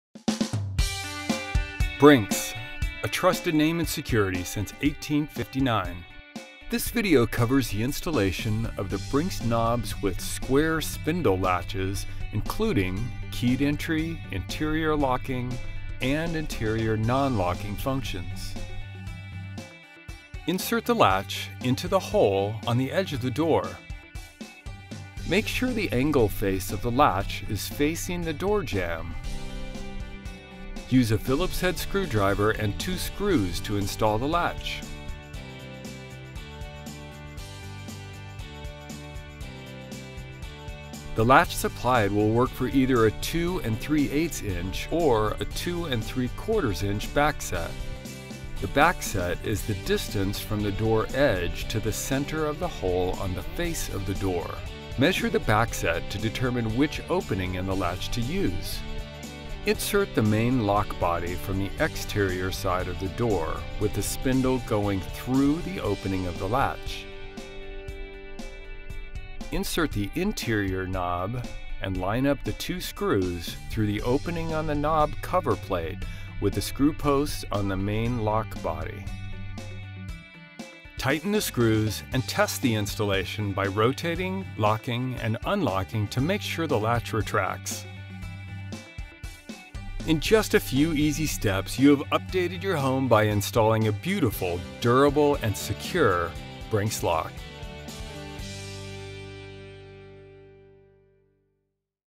Brink's Blue Series Square Spindle Keyed Entry Knob Installation
Watch how easy it is to install a Brink's Blue Series Keyed Entry Knob in your home. Installation normally requires only a few minutes of your time and will update your home with a beautiful, durable and secure Brink's Lock.